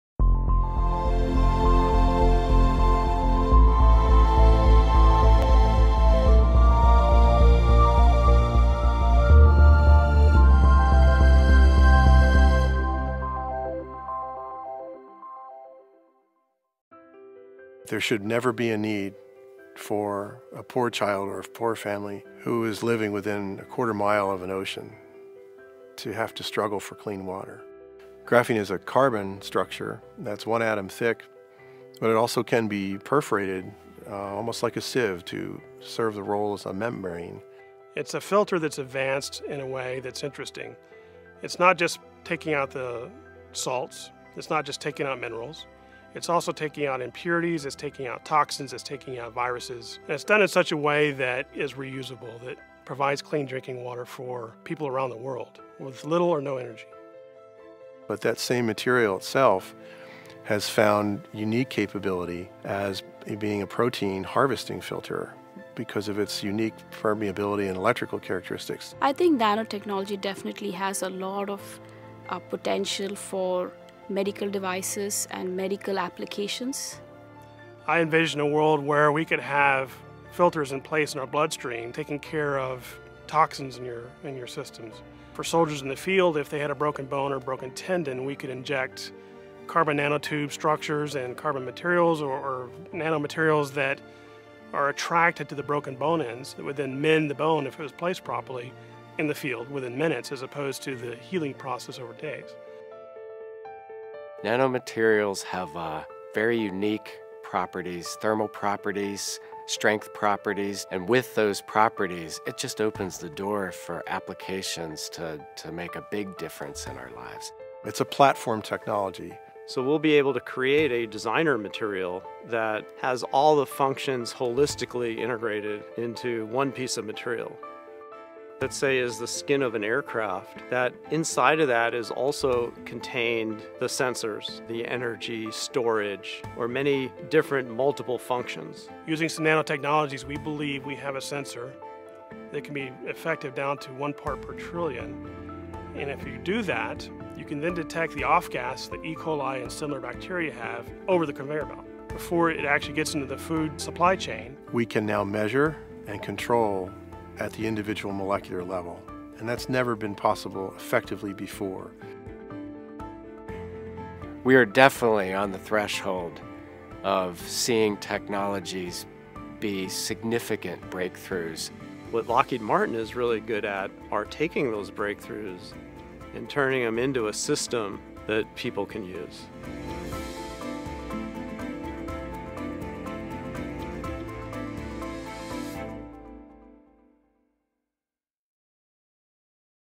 Let's talk about the Future: Nanotechnology
Explore Nanotechnology - Imagine a world where unique phenomena at the molecular scale can lead to entirely new, innovative, and transformative product designs—all done by harnessing properties of materials at the nanoscale level. Nanotechnology will revolutionize the way we engineer new technologies, impacting areas of next-generation energy storage, high-performance electronics and multi-functional materials.